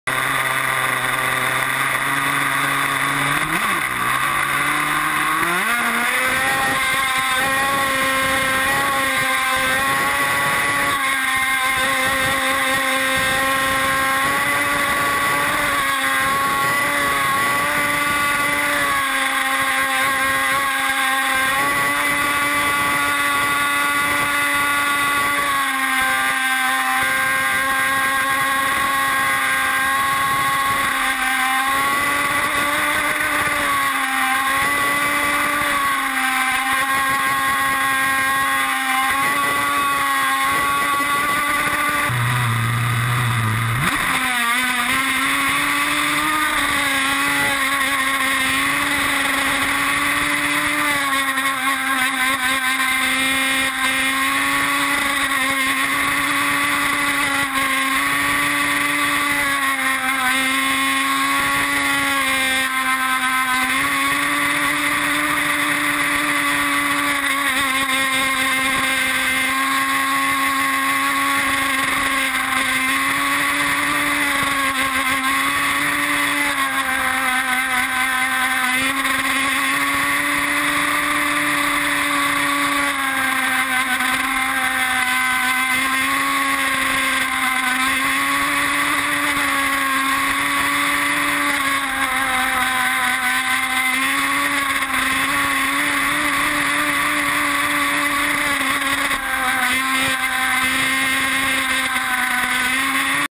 fathers day run - R42
Here is one of my R42s running today on the first day of Spring